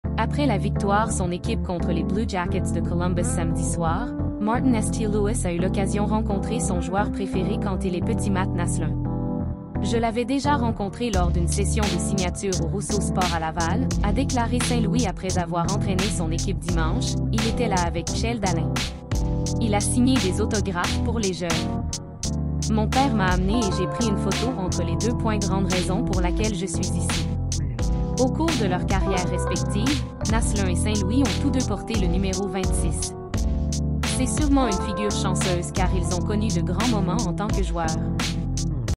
Mats Naslund: une inspiration pour Martin St-Louis
Après la victoire son équipe contre les Blue Jackets de Columbus samedi soir, Martin St-Louis a eu l'occasion rencontrer son joueur préféré quand il est petit Mats Naslund. "Je l'avais déjà rencontré lors d'une session de signature au Rousseau Sports à Laval", a déclaré St-Louis après avoir entraîné son équipe dimanche  «Il était là avec Kjell Dahlin. Il a signé des autographes pour les jeunes. Mon père m'a amené et j'ai pris une photo entre les deux.grande raison pour laquelle je suis ici. Au cours de leur carrière respective, Naslund et St-Louis ont tous deux porté le numéro 26. C'est sûrement une figure chanceuse car ils ont connu de grands moments en tant que joueurs.